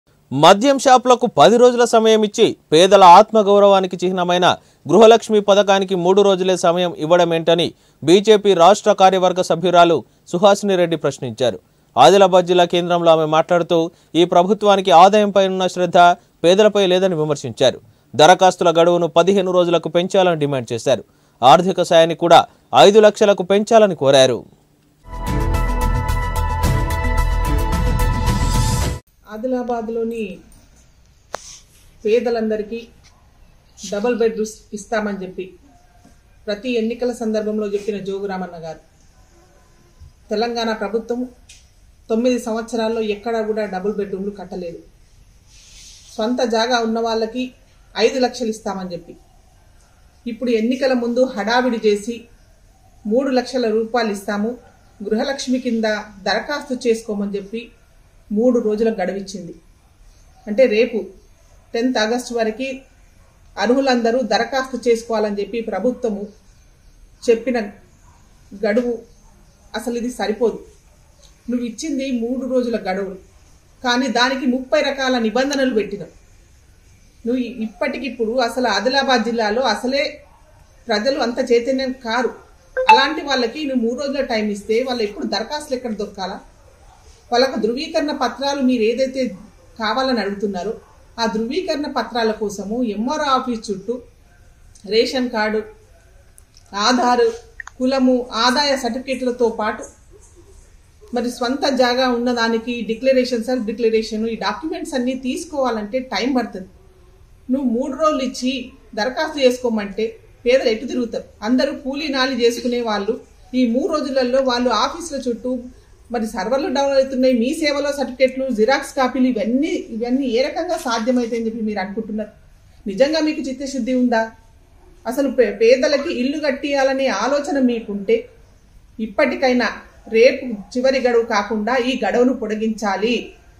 గృహలక్ష్మి పథకానికి దరఖాస్తుల గడవు 15 రోజులకు పెంచాలని డిమాండ్
గృహలక్ష్మి పథకానికి దరఖాస్తుల గడవు 15 రోజులకు పెంచాలని డిమాండ్ || Demand to increase the application deadline for Grilahakshmi scheme to 15 days ||@S6News

మద్యం షాపులకు 10రోజుల సమయం ఇచ్చి పేదల ఆత్మగౌరవానికి చిహ్నమైన గృహలక్ష్మి పథకానికి 3రోజులే సమయం ఇవ్వడమేంటని బీజేపీ రాష్ట్ర కార్యవర్గ సభ్యురాలు సుహాసినిరెడ్డి ప్రశ్నించారు.// ఆదిలాబాద్ జిల్లా కేంద్రంలో ఆమె మాట్లాడుతూ ఈ ప్రభుత్వానికి ఆదాయంపై ఉన్న శ్రద్ధ పేదలపై లేదని విమర్శించారు.// దరఖాస్తుల గడవును 15రోజులకు పెంచాలని డిమాండ్ చేశారు.// ఆర్థికసాయాన్ని కూడా 5లక్షలకు పెంచాలని కోరారు.//